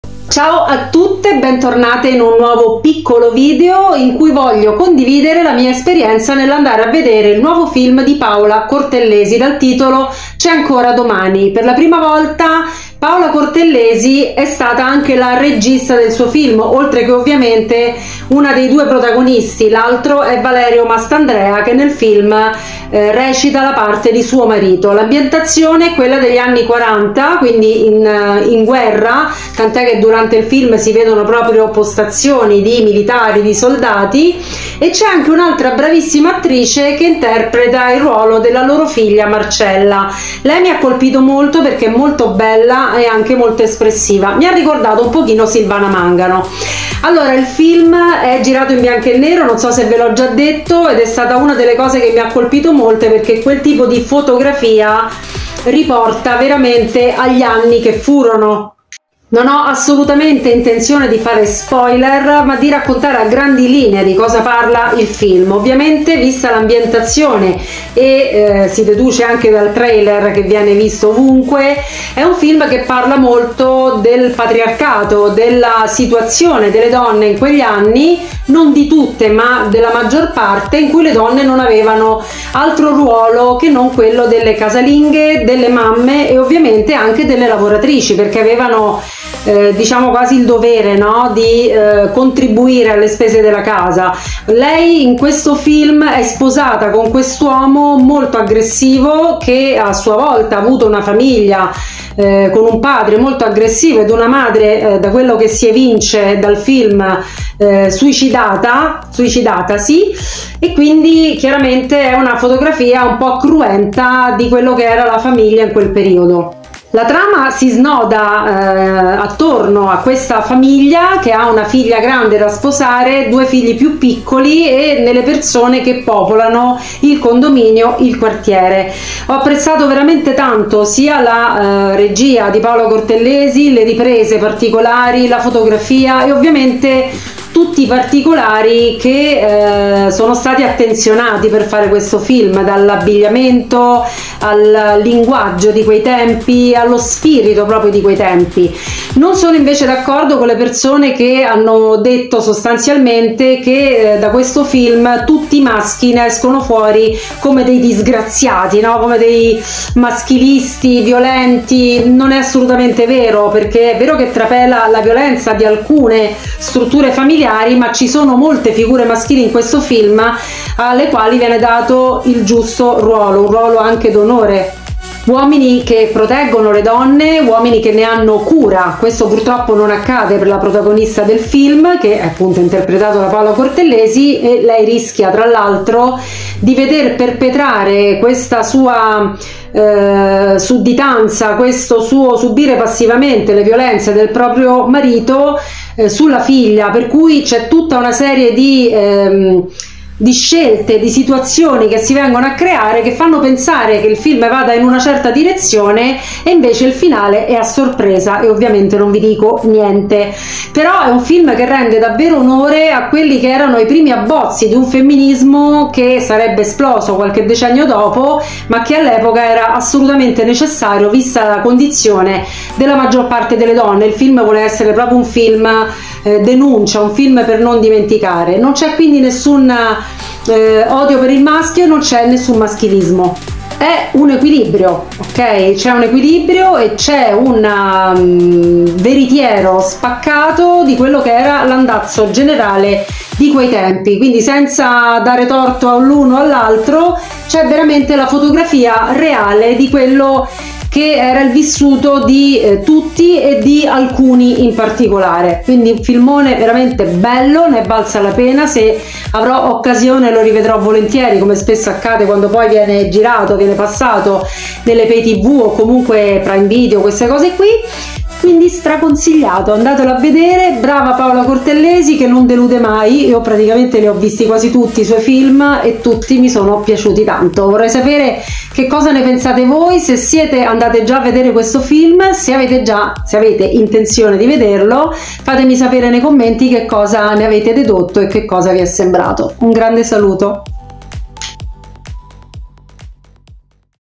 C'È ANCORA DOMANI
Il film 🎥  sotto la regia, per la prima volta, di PAOLA CORTELLESI, con Valerio Mastandrea, volutamente girato in bianco e nero per meglio far identificare lo spettatore con la realtà  vissuta attorno al 1945.

Lo aspettavo con fervore da settimane e ho acquistato in anticipo i biglietti il giorno prima dell'uscita.

Le mie aspettative non sono state deluse: oltre alla bravura magistrale dei due attori protagonisti, che hanno saputo ben impersonare il patriarcato di quegli anni e l'abbozzo delle prime spinte del femminismo per conquistarsi un pezzo dei diritti, della dignità e del rispetto necessario alle donne dell'epoca, mi ha colpito molto anche la bravura della giovane attrice che interpreta la figlia primogenita (Romana Maggiora Vergaro), che mi ha vagamente ricordato Silvana Mangano, poichè molto espressiva.

La Cortellesi ha saputo narrare alcune crudezze dei sistemi familiari dell'epoca nel ceto basso, condendole al contempo della scanzonata leggerezza che la  contraddistingue in tutte le pellicole in cui l'abbiamo vista protagonista, con quel suo miscuglio di malinconica ironia negli sguardi torvi e nei sorrisi mesti.

È la lotta di una madre per il futuro di sua figlia, e di tutte le donne.

Niente spoiler, solo la raccomandazione di non perdervi questo piccolo capolavoro, anche per la sua colonna sonora.